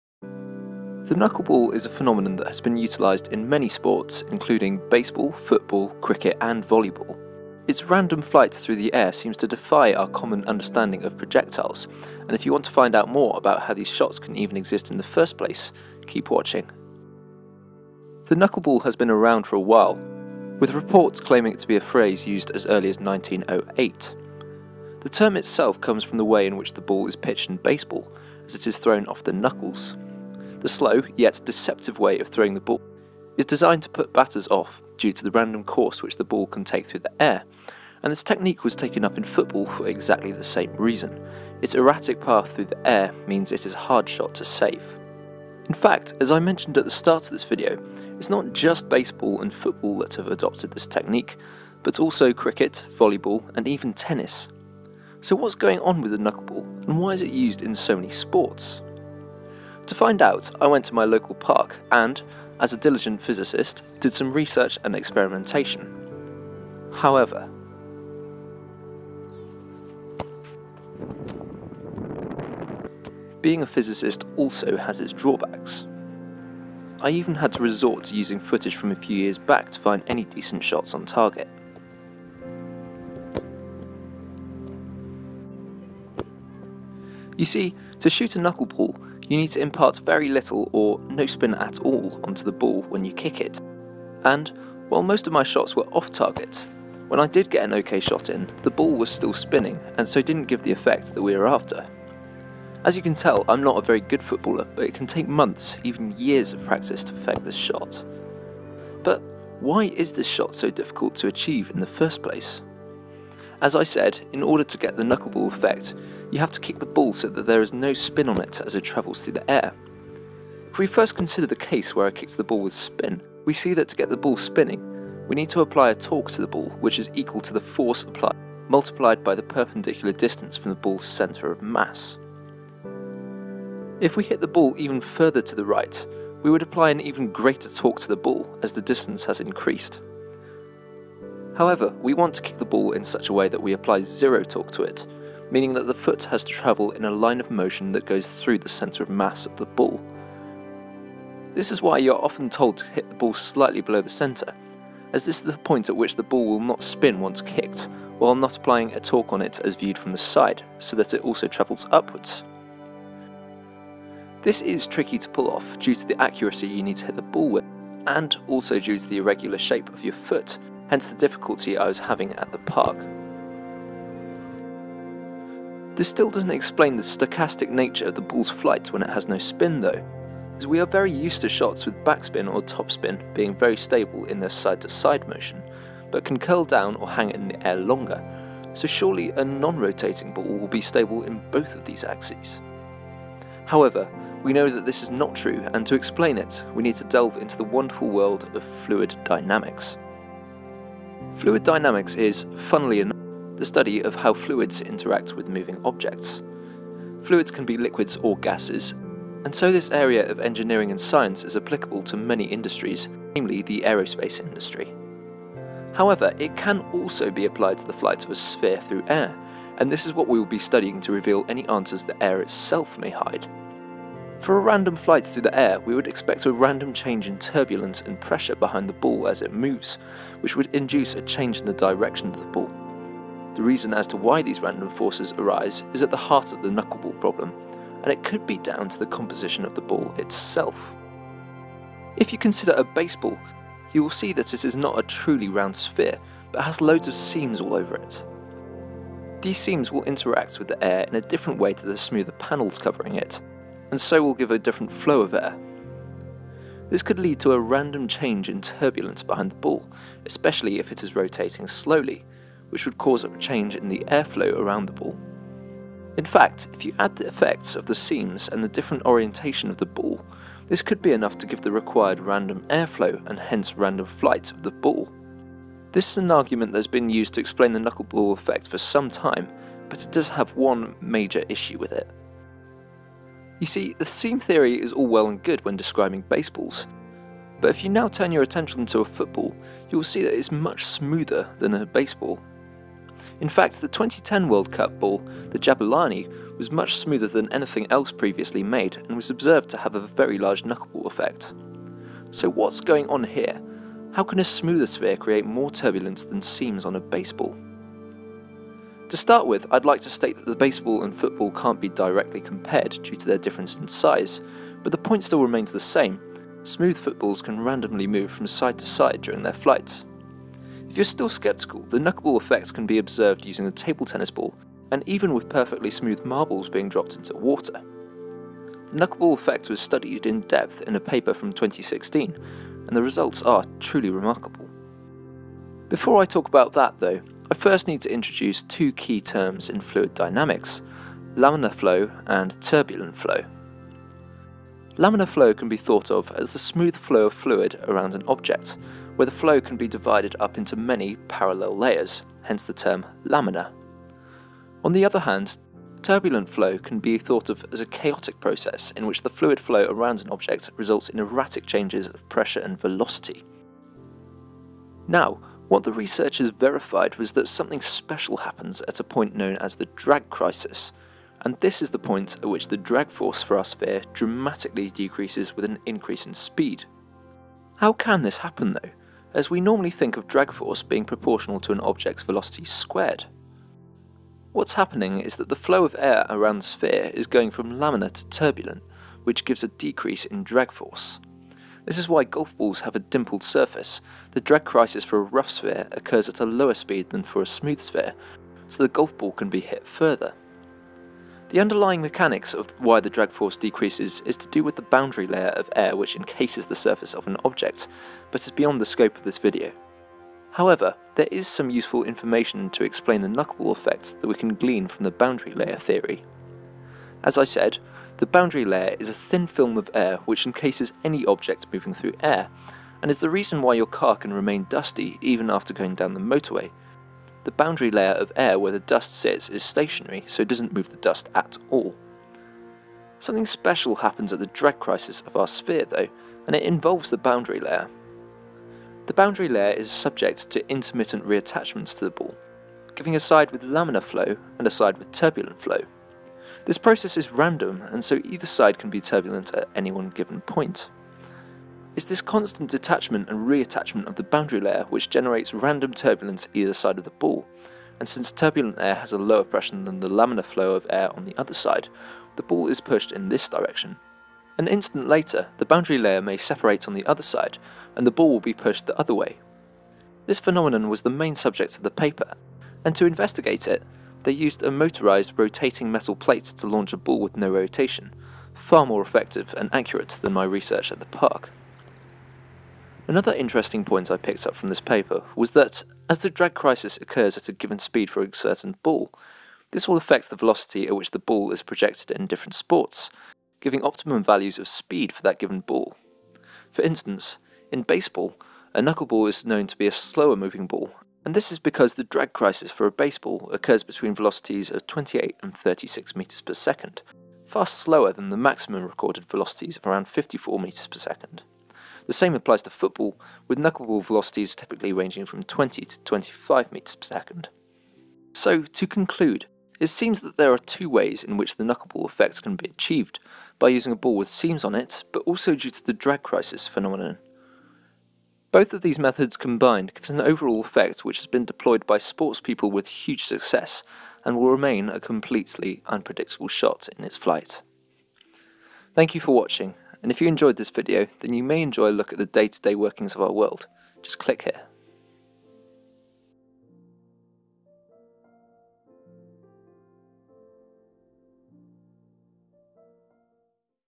How does a Knuckleball Work? The Science of the Knuckleball
How does a knuckleball work, and what is a knuckleball? In this video I'll be explaining the science behind the knuckleball, a shot utilised by baseball players and footballing greats such as Ronaldo and Bale, and how this shot can possiblly work in the first place.

The knuckleball is a shot that has confused goalkeepers and batters alike for decades, and the underlying science of the knuckleball is really quite amazing. But how does a knuckleball work, and what is a knuckleball in soccer, or football?

One of the earlier theories that tried to explain how a knuckleball works was based on the fact that the balls used in baseball are covered in seams, and so the knuckleball effect would arise due to the turbulence that these seams created. This fails to explain how a knuckleball works in table tennis and football, as these sports use much smoother balls.

How does a knuckleball work without seams on the ball? The answer to this is down to the boundary layer theory, and the fact that a knuckleball effect is observed at a velocity known as the drag crisis. The drag crisis occurs when the air flow past the ball goes from laminar to turbulent, and it's this that gives the desired knuckleball effect. The boundary layer detaches from the ball intermittently on one side, causing a side to have turbulent flow behind it, and can then reattach and detach on the other side. This random process gives rise to a ball that can swerve randomly through the air, which is a knuckleball.

The science behind the knuckleball can be very complicated, but I hope that in this video, I can simplify this phenomenon and deliver an explanation as to how a knuckleball works in more simplified terms.